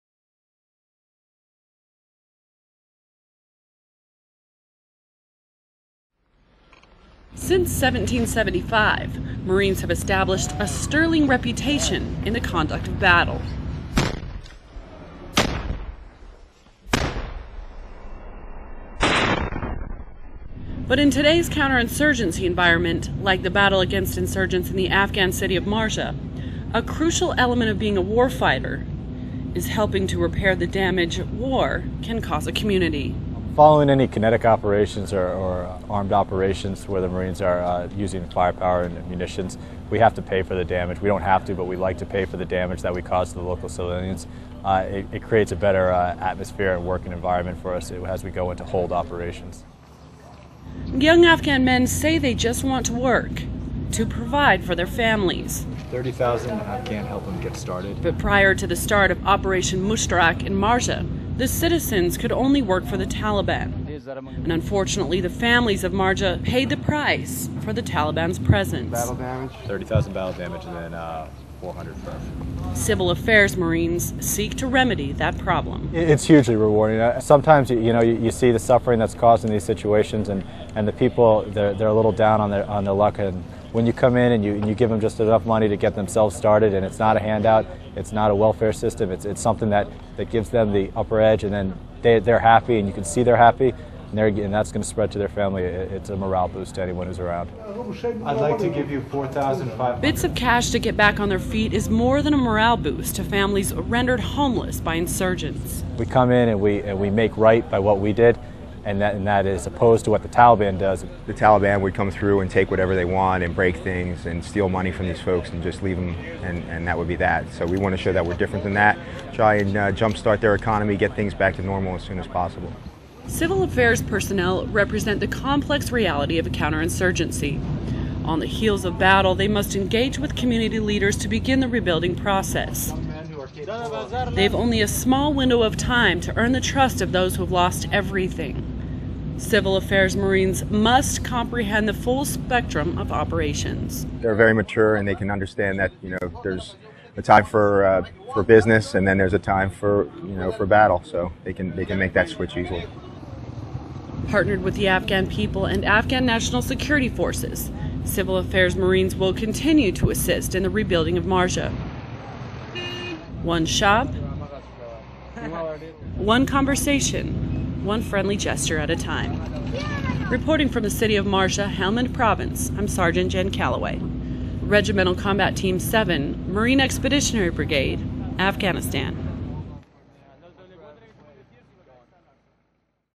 Marines repair battle-damaged city in Afghanistan
Civil Affairs Marines mission is to make amends with innocent villagers who's livelihood has been affected by battle. Watch the process first-hand.